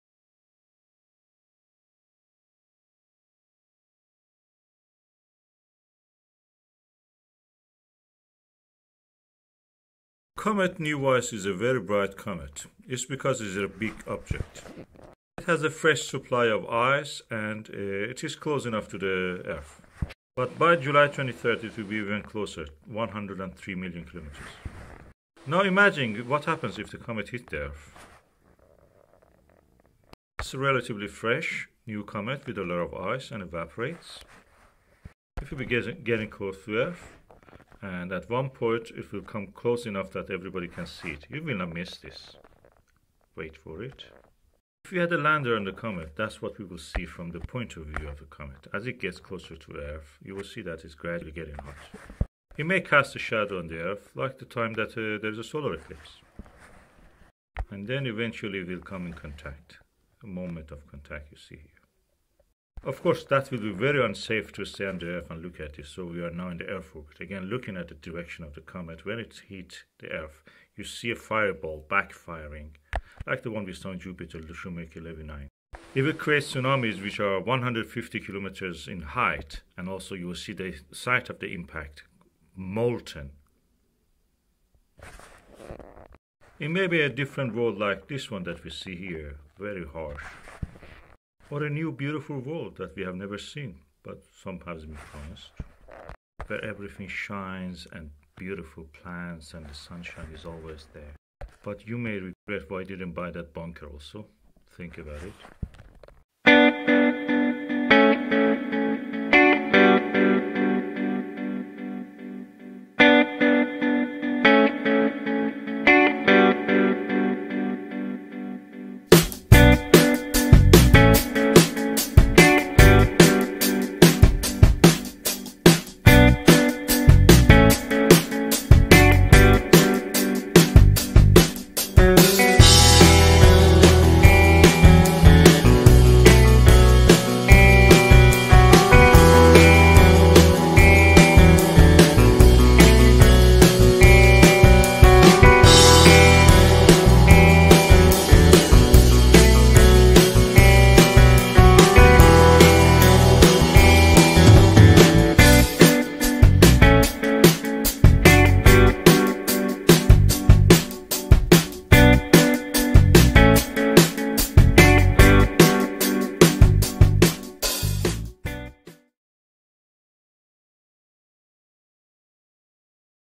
Buy Bunker Before Comet NEOWISE Hit The Earth?/ Apocalypse, Doomsday
End time, is a time period desired and promised in the eschatological writings in Israelite religions and in doomsday cults, wishing a Global catastrophe to bring an end to the world. It is a hypothetical event explored in science and fiction where human civilization or life is at risk of partial or complete destruction.
C/2020 F3 (NEOWISE), or Comet NEOWISE, is a retrograde comet with a near-parabolic orbit discovered on March 27, 2020 by the NEOWISE space Telescope. It passed closest to the Sun on July 3, 2020. As of 10 June 2020 it was apparent magnitude 7,  and if it survives perihelion 0.29 AU (43 million km; 110 LD) from the Sun, it is expected to be visible to the naked eye in July. When the comet entered the field of view of SOHO LASCO C3 on 22 June 2020 the comet had brightened to magnitude 3. As of 1 July, Comet NEOWISE has brightened to magnitude +1, with light curve observations suggesting it might reach Magnitude - 2.0 at perihelion, far exceeding the brightness attained by C/2020 F8 (SWAN), and reports indicate the comet developed a second tail. One tail is made of dust and the other tail is made of gas.

Planet Venus is at its highest position in Sky. The Nibiru cataclysm is a supposed disastrous encounter between the Earth and a large planetary object (either a collision or a near-miss) that certain groups believed would take place in the early 21st century. Believers in this doomsday event usually refer to this object as Nibiru or Planet X. The idea was first put forward in 1995 by Nancy Lieder, founder of the website ZetaTalk.

Many comets and asteroids collided with Earth in its early stages. Many scientists think that comets bombarding the young Earth about 4 billion years ago brought the vast quantities of water that now fill Earth's oceans, or at least a significant portion of it. Others have cast doubt on this idea. The detection of organic molecules, including polycyclic aromatic hydrocarbons, in significant quantities in comets has led to speculation that comets or meteorites may have brought the precursors of life—or even life itself—to Earth.In 2013 it was suggested that impacts between rocky and icy surfaces, such as comets, had the potential to create the amino acids that make up proteins through shock synthesis.